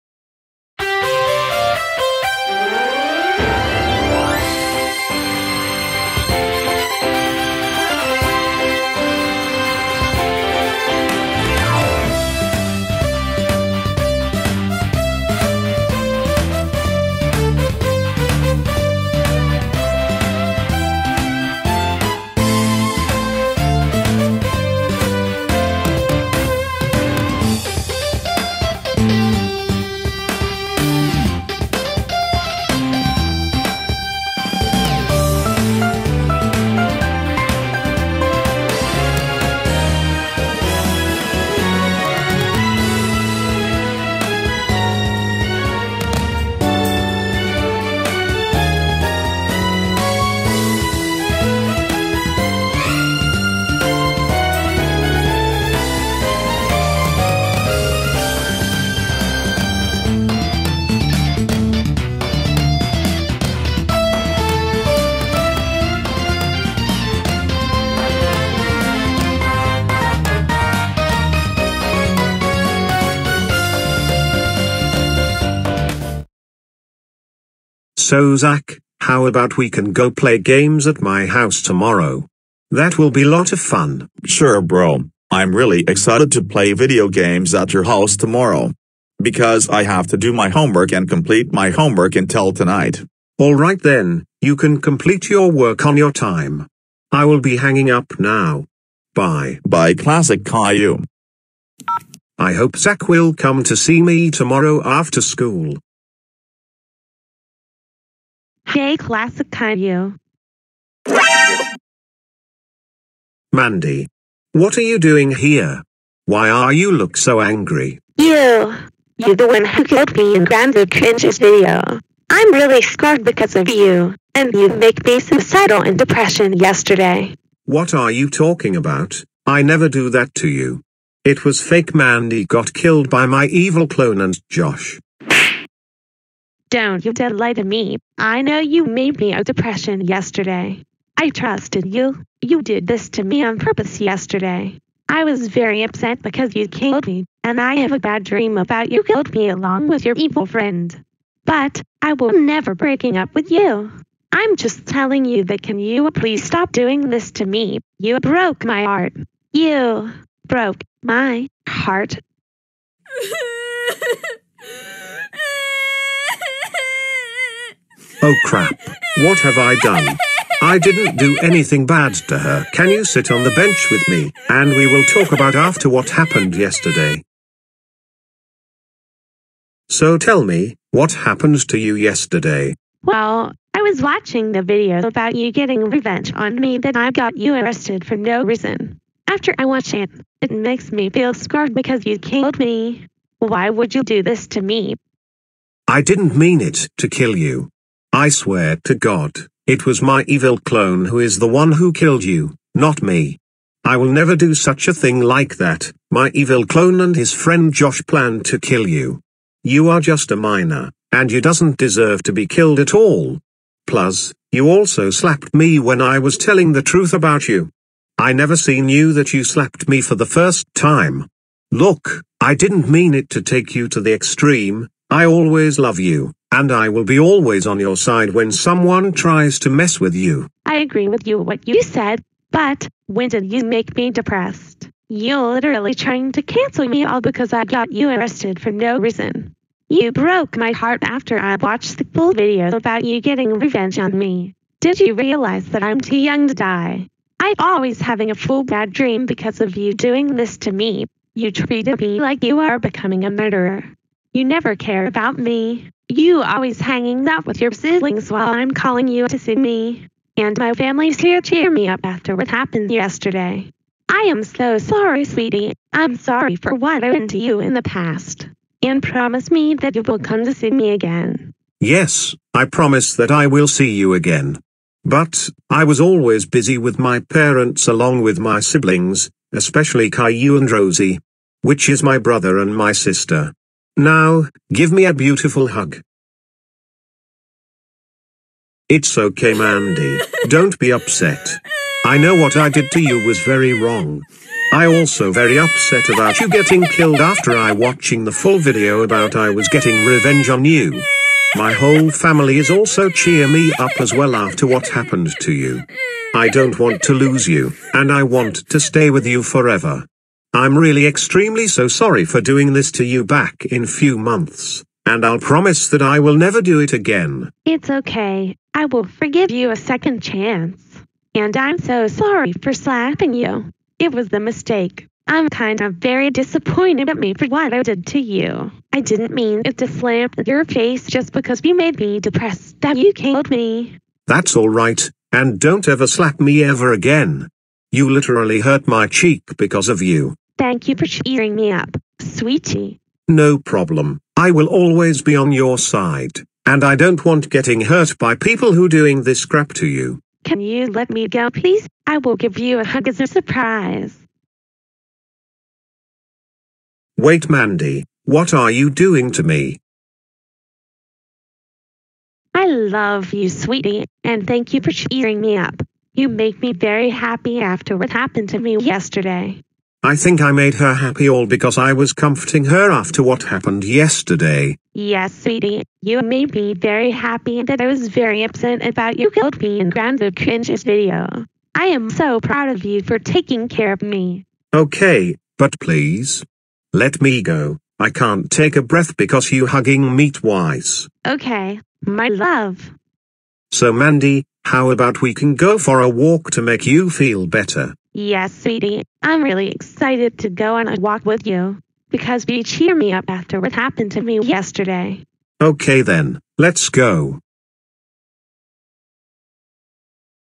Classic Caillou Hugs Mandy While She Is Crying
Mandy is not happy when Classic Caillou made her s dal and depression because she was very scared about Classic Caillou killed her in the previous video.